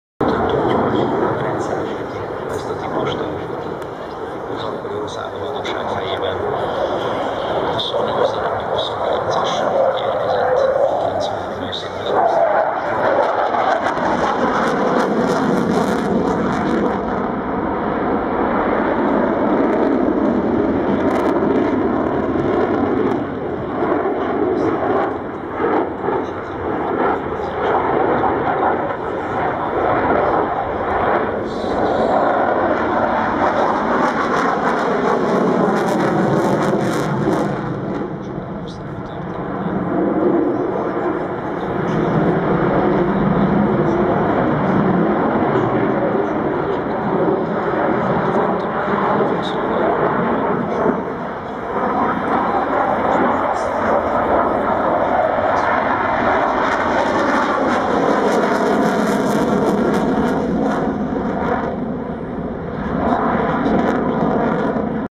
Mikoyan Gurevich Mig 29 is high speed turning twice at Kecskemét Air Show 2010 Exclusive
Mikoyan Gurevich Mig 29 is high speed turning twice at Kecskemét Air Show 2010
Check my other videos of the air show